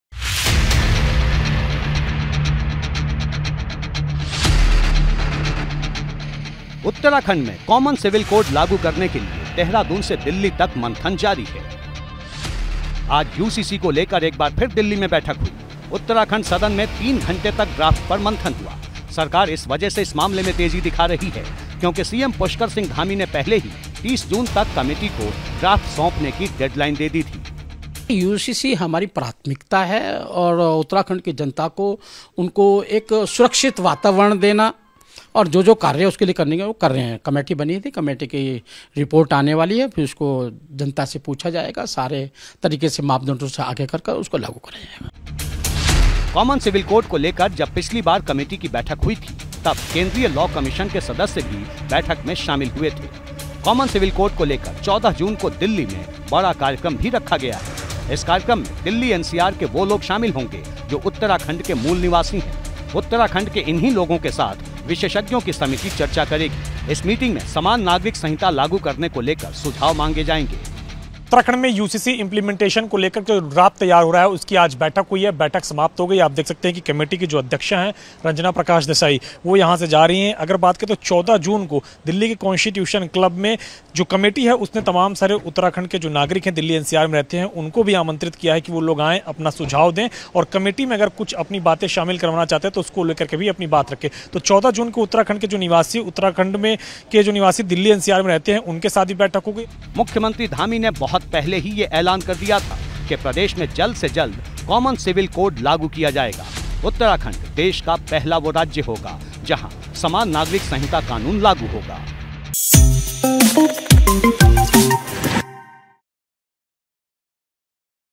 Common Civil Code पर तेजी से काम कर रही Pushkar Singh Dhami की सरकार !
News: बात उत्तराखंड की... जहां की धामी सरकार... कॉमन सिविल कोड को लेकर तेजी से काम में जुटी है। मुख्यमंत्री पुष्कर सिंह धामी पहले ही कमेटी को ड्रॉफ्ट सौंपने के लिए 30 जून तक की डेडलाइन दे चुके हैं। आज कमेटी ने दिल्ली में ड्राफ्ट को लेकर अहम बैठक की... वहीं 14 जून को इस मुद्दे पर दिल्ली में एक अहम मीटिंग होने वाली है... जिसमें उत्तराखंड के भी लोग शामिल होंगे... और समान नागरिक संहिता को लेकर सुझाव देंगे...